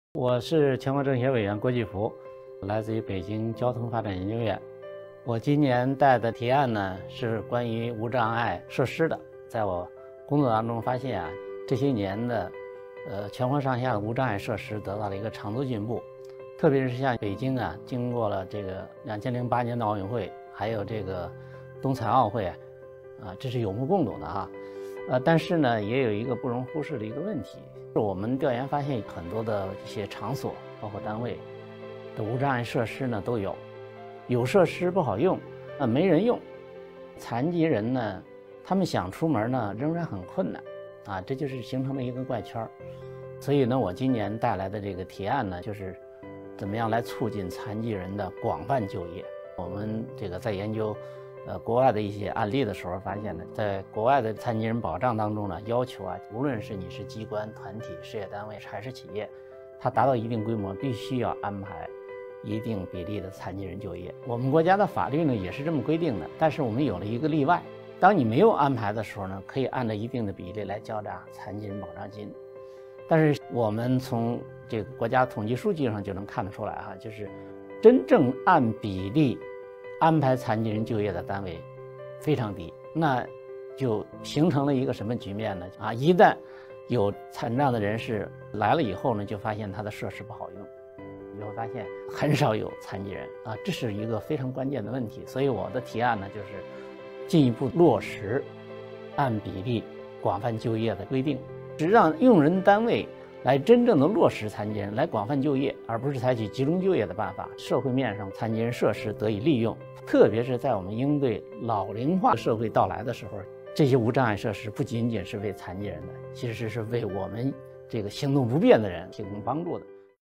【2023看两会】郭继孚委员：落实残疾人按比例广泛就业 提高无障碍设施利用率
全国政协委员、北京交通发展研究院院长郭继孚表示，近年来，中国无障碍环境建设取得了长足的进步。但据调研结果显示，根据规模按比例安排残疾人就业的单位少之又少，同时存在用人单位无障碍设施使用感不佳、使用率不高等问题，导致残疾人无法就业或只能集中就业。对此，他建议进一步落实按比例广泛就业的规定，让用人单位真正落实政策，无障碍设施能够得到充分利用。